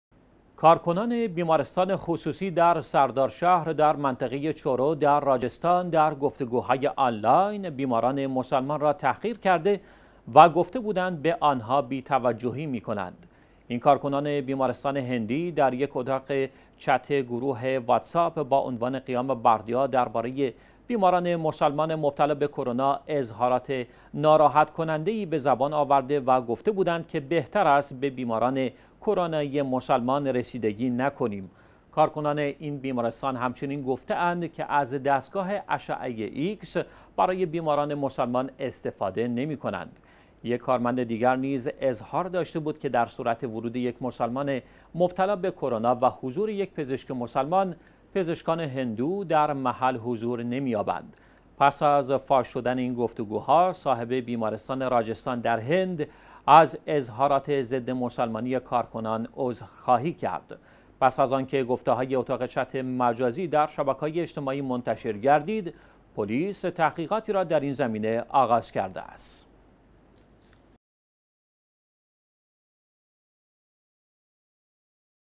اظهارات کارکنان بیمارستان هند در عدم معالجه بیماران مسلمان
کارکنان بیمارستان خصوصی در سردارشهر در منطقه چورو در راجستان در گفتگوهای آنلاین، بیماران مسلمان را تحقیر کرده و گفته بودند به آن‌ها بی توجهی می‌کنند.
این کارکنان بیمارستان هندی در یک اتاق چت گروه واتس آپ با عنوان «قیام بردیا» درباره بیماران مسلمان مبتلا به کرونا اظهارات ناراحت کننده‌ای به زبان آورده و گفته بودند که بهتر است به بیماران کرونایی مسلمان رسیدگی نکنیم.
کارکنان این بیمارستان همچنین گفته اند که از دستگاه اشعه ایکس برای بیماران مسلمان استفاده نمی‌کنند. یک کارمند دیگر نیز اظهار داشته بود که در صورت ورود یک مسلمان مبتلا به کرونا و حضور یک پزشک مسلمان، پزشکان هندو در محل حضور نمی‌یابند.
پس از فاش شدن این گفتگوها، صاحب بیمارستان راجستان در هند از اظهارات ضدمسلمانی کارکنان عذرخواهی کرد.
پس از آنکه گفته های اتاق چت مجازی در شبکه‌های اجتماعی منتشر گردید، پلیس تحقیقاتی را در این زمینه آغاز کرده است.